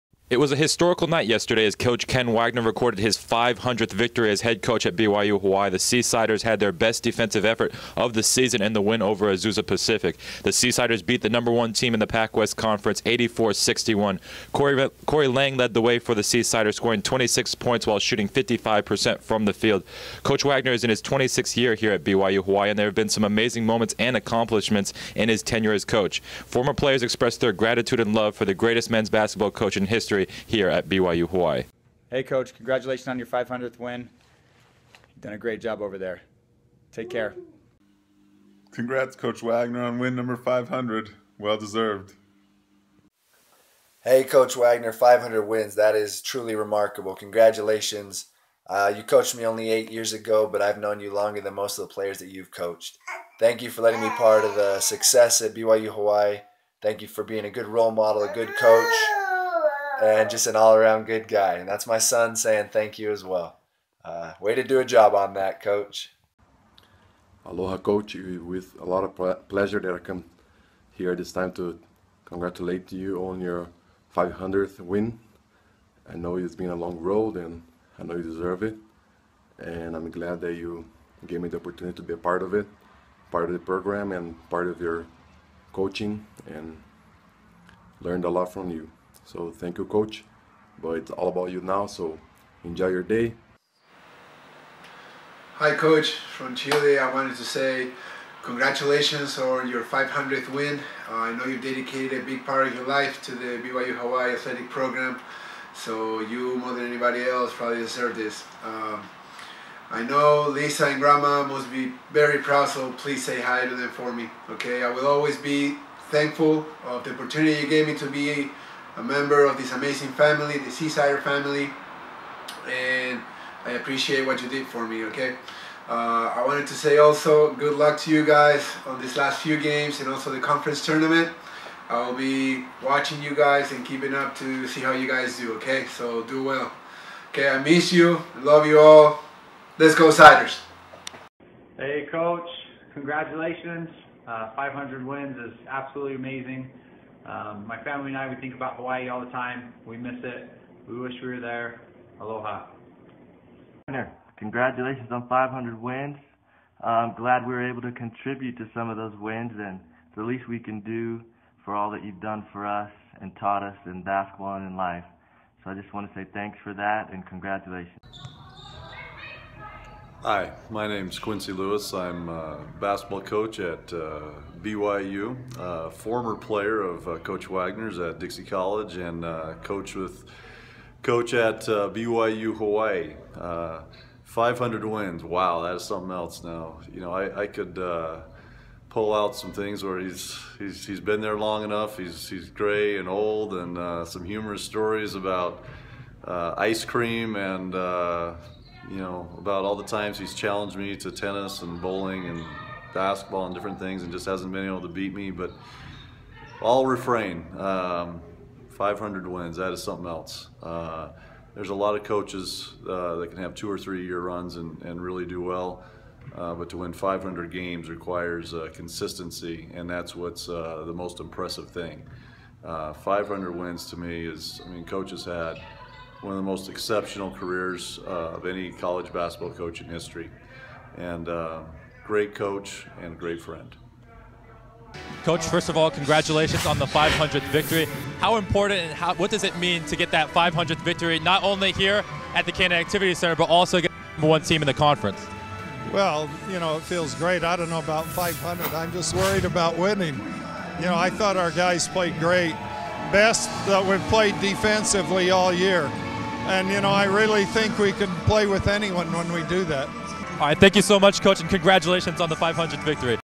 Ken Wagner's 500th
Ken Wagner recorded his 500th victory as head coach at BYU-Hawaii. Former players show their respect to the Seasider coach.